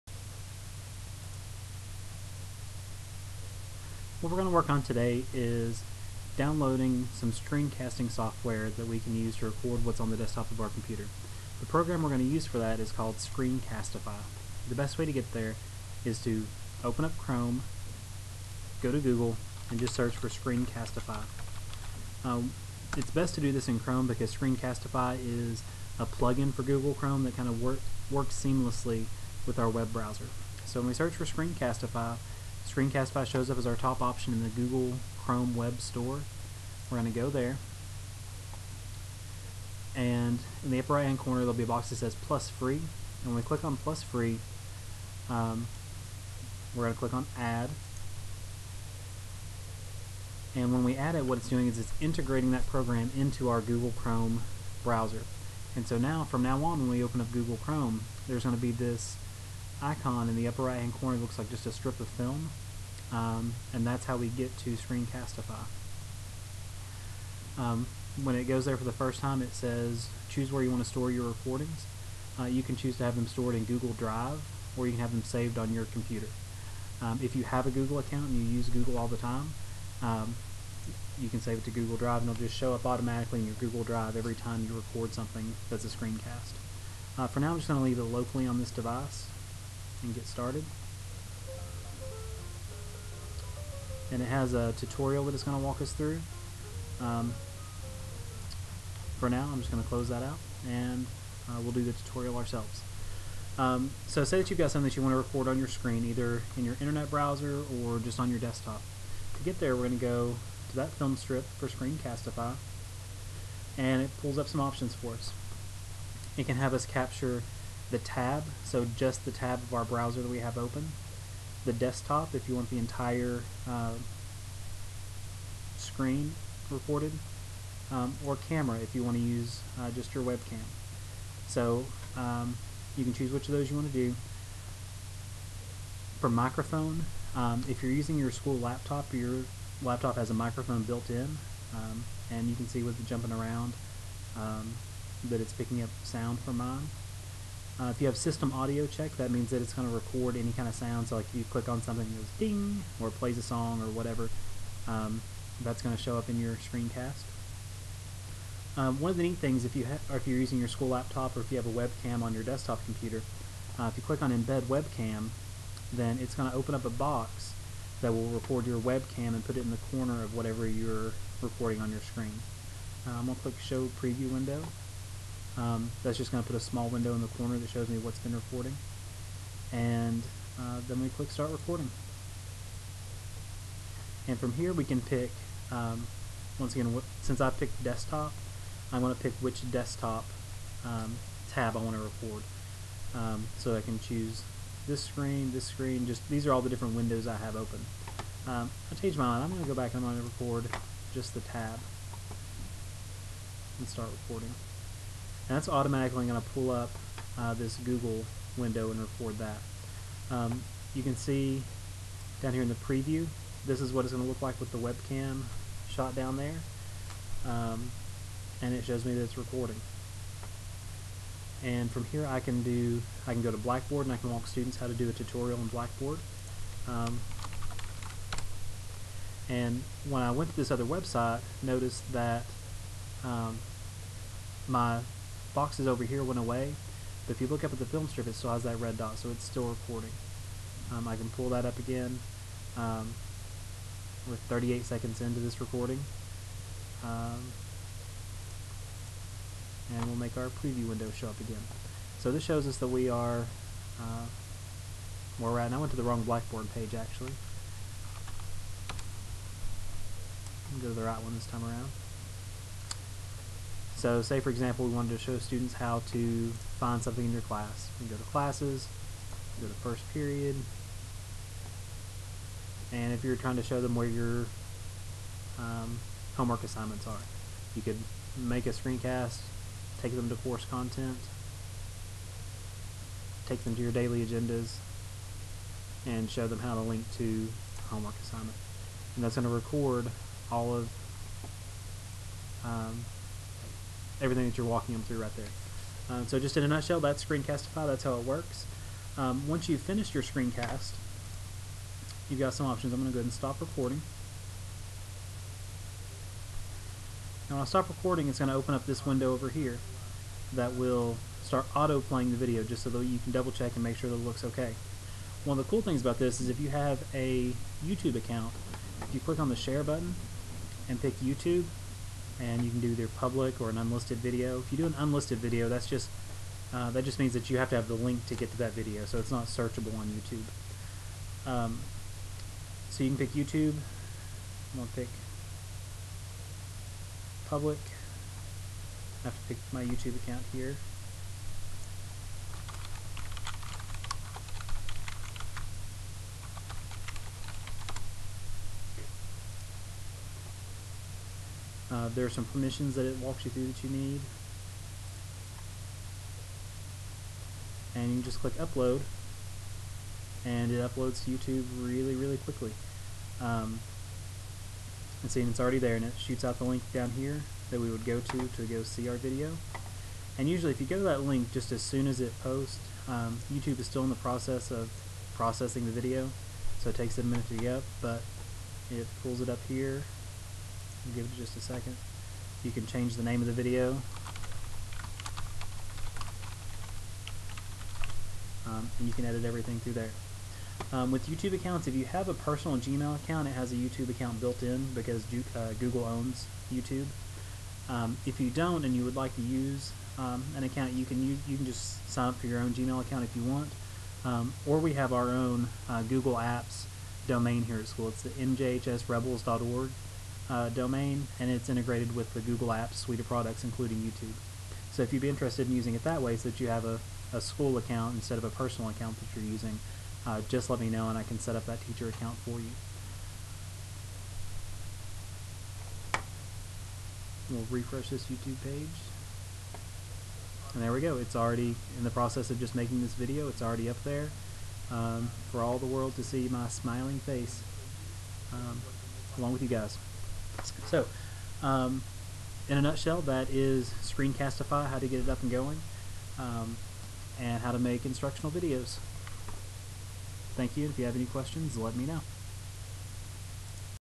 Downloading and Using Screen Castify
Ironically, this video about Screen Castify had to be made with another screen recorder in order to be able to record the Screen Castify dialog boxes. Since then, I've figured out how to record those dialog boxes (capture Desktop instead of just the Tab).